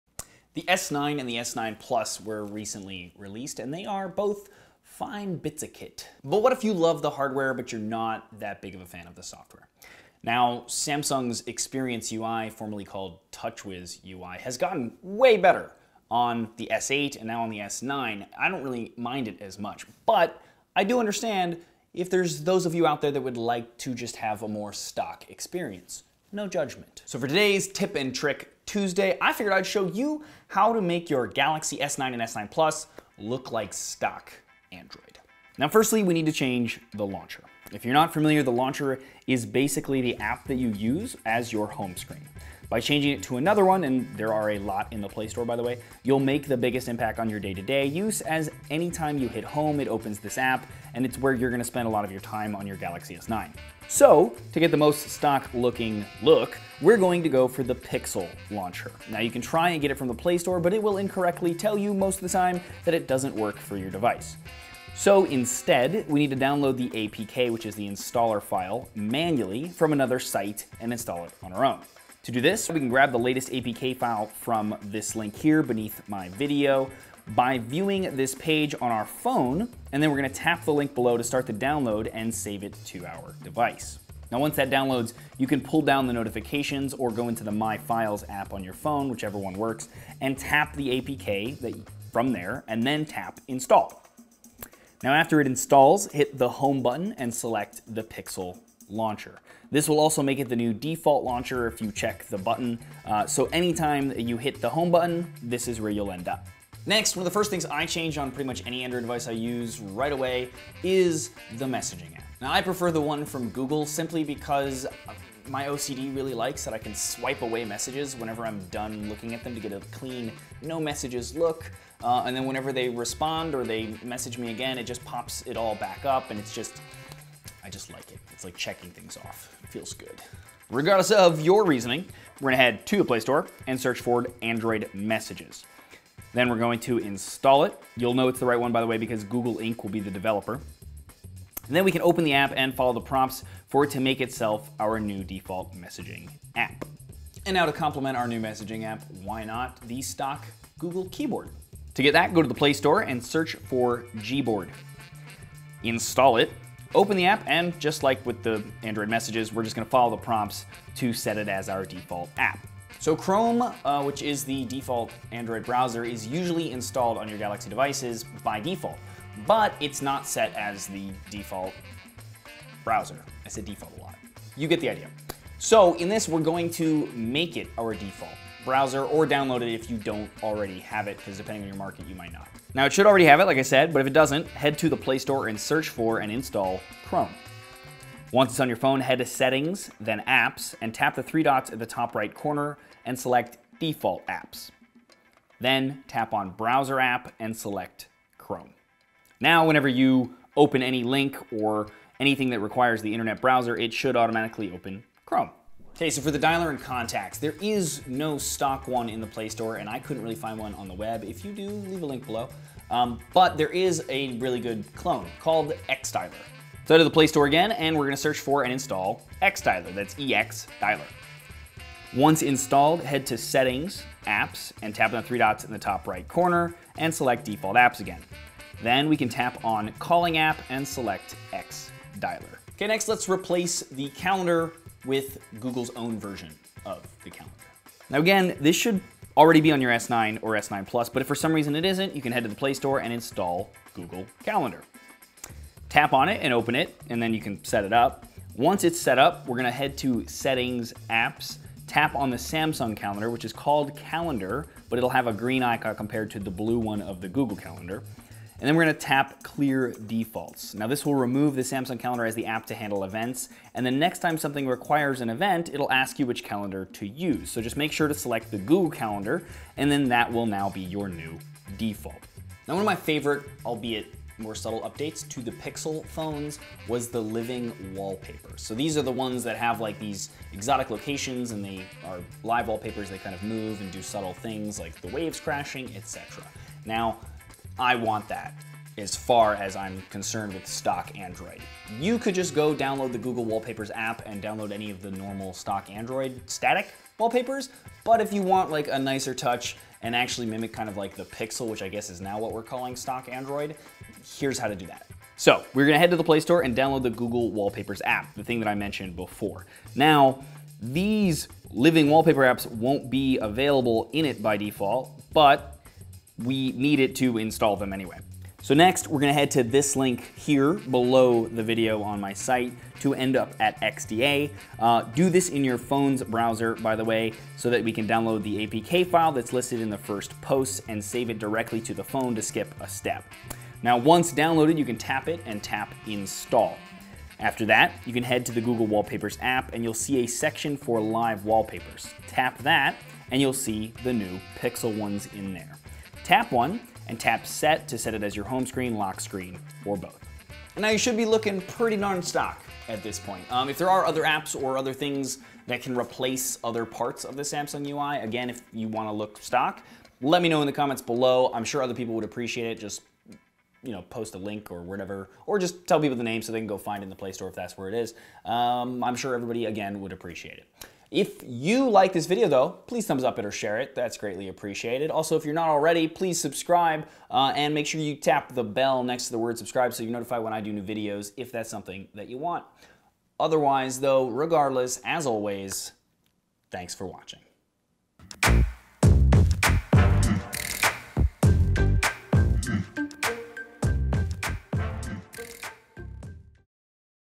How to Make the Samsung Galaxy S9 Look Like Stock Android
The S9 and S9 Plus were released recently and they are a fine bit of kit. They feel more solid than the S8 and S8 Plus for some reason, the low-light camera is insane, and that screen? Drool.

But what if you love the hardware but aren't the biggest fan of the software or Samsung's Experience UI (formerly TouchWiz UI). Now, I admit Samsung's UI is way better than it used to be, but some people out there might want to use stock Android instead--no judgement, I get it. So in this tip and trick Tuesday, here's how to make the Galaxy S9 or S9 Plus look like stock Android.